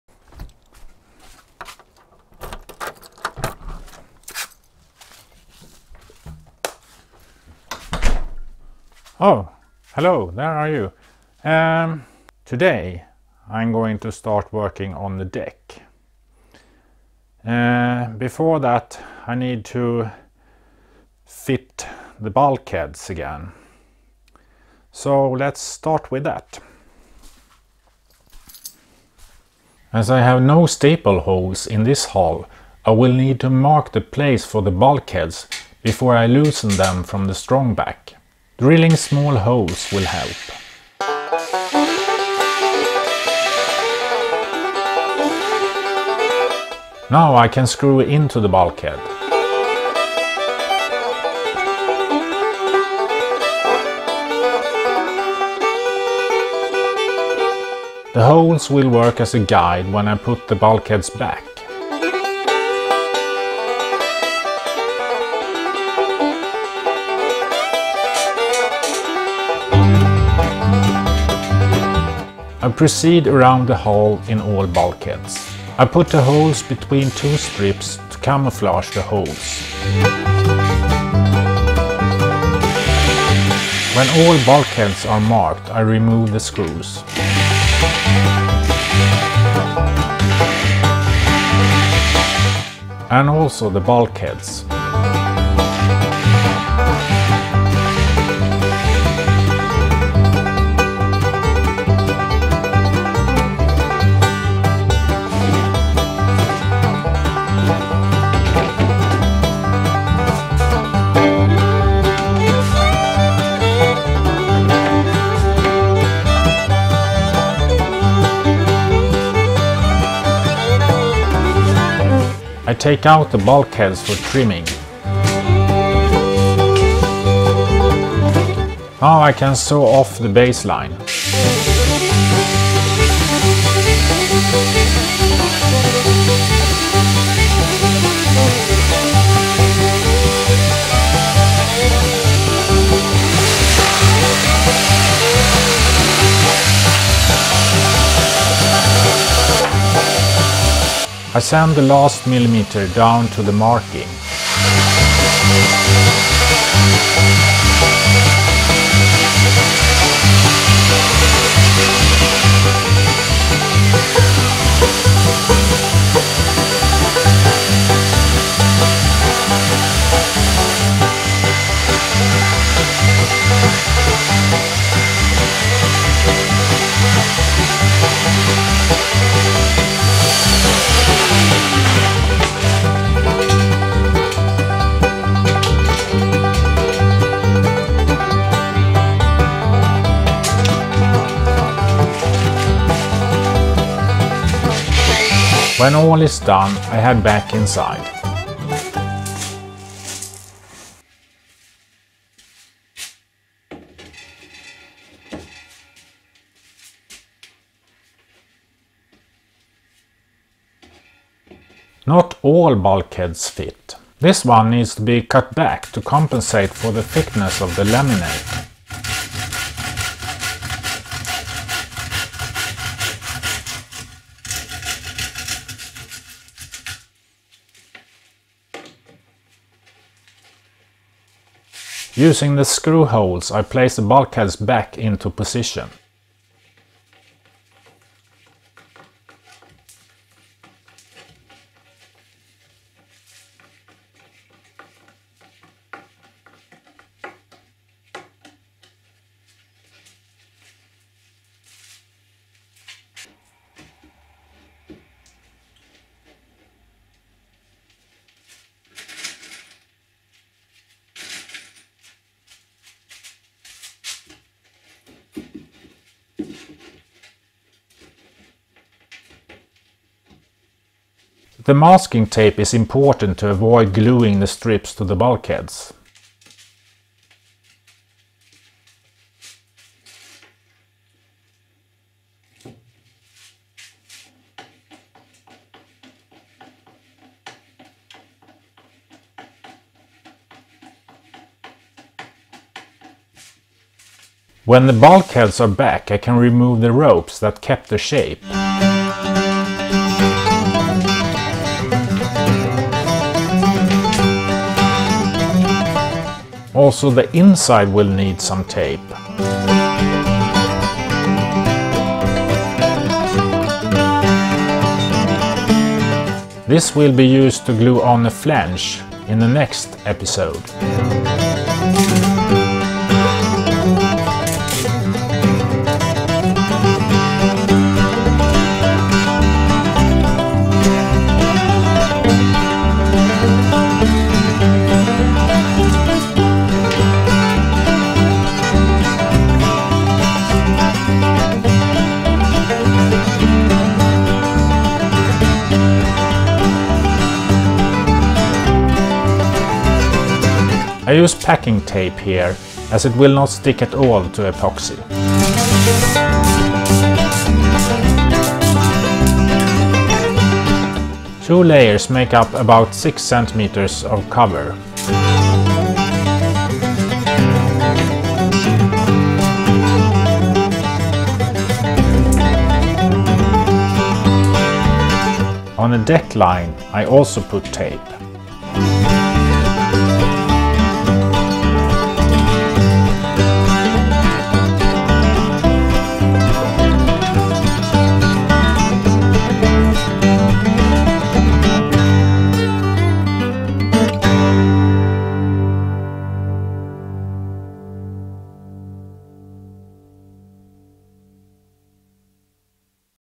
Fitting the bulkheads back into the hull in preparation for laying the deck on the DIY Kayak. #112
In this video I measure out where to place the bulkheads, cut them to the blueprint and fit them in to the hull so I later can start on the deck. As I have no staple holes to follow I need to figure out another way to place them correctly.